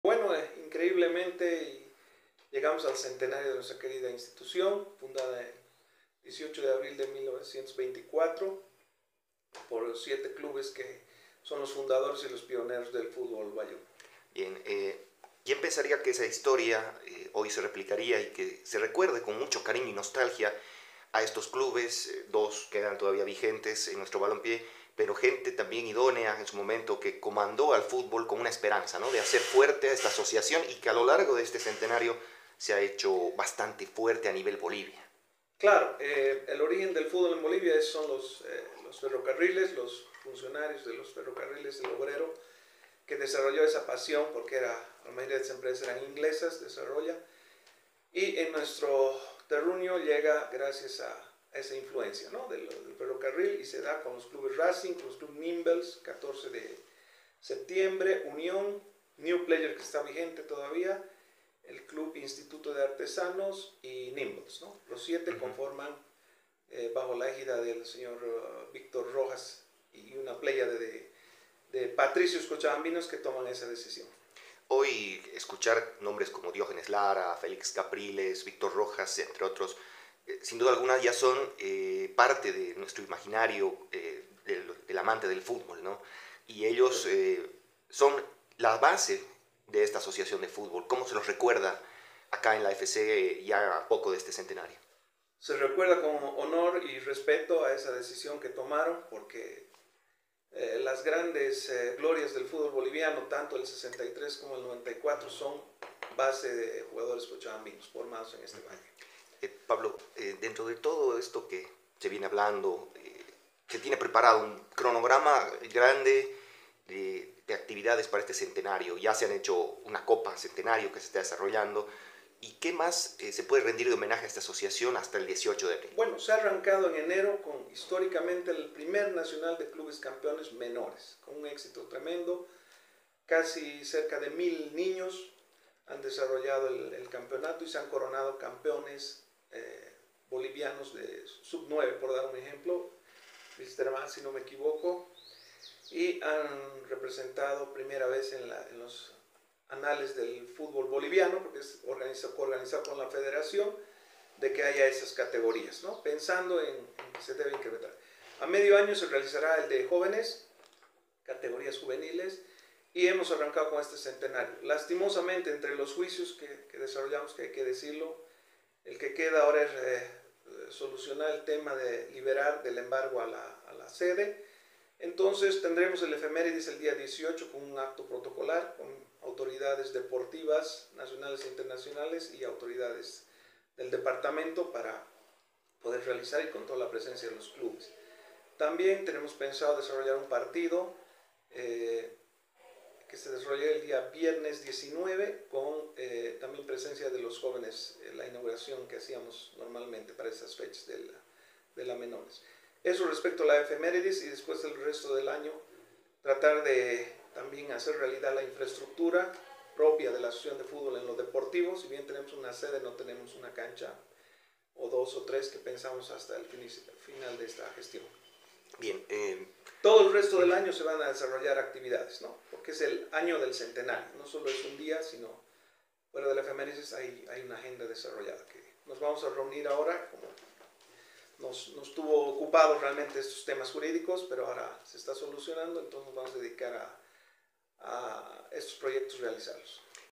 Asociación de Fútbol Cochabamba, el génesis del nacimiento de la pasión
El próximo jueves 18 de abril, la Asociación de Fútbol Cochabamba (AFC) celebrará 100 años de fundación, un centenario cargado de muchas historias, anécdotas, sueños y objetivos aún por cumplir. La asociación es la génesis del balompié valluno, donde los clubes históricos y más queridos de la región vieron  la luz, donde se forjaron a los jugadores más valiosos del fútbol nacional y que se volvió en una verdadera cuna de campeones.